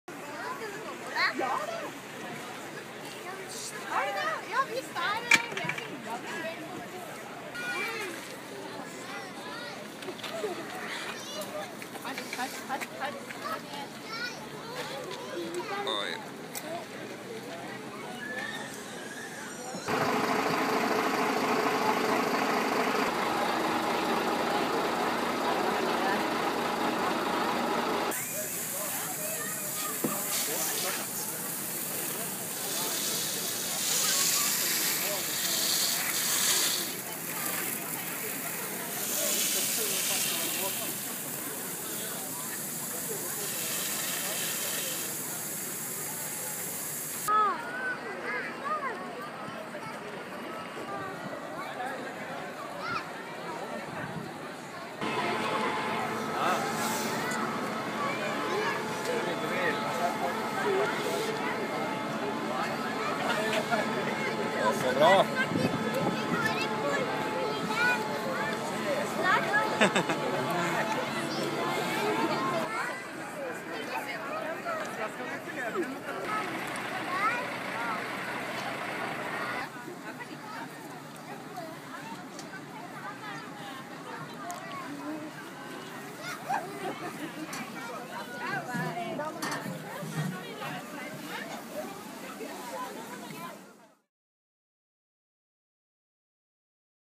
Tobias på Brannvernuka 2012
Tobias på åpen dag på Hamar brannstasjon 22. september 2012.